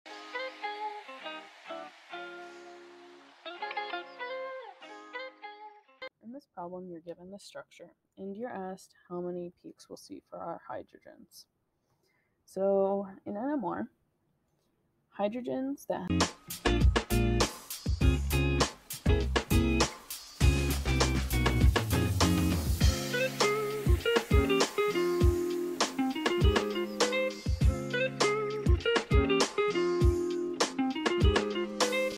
Testbank Question 20 For the following compound, how many different signals would you see in the pr…
Testbank Question 20 For the following compound, how many different signals would you see in the proton NMR? (Assume that you can see them all)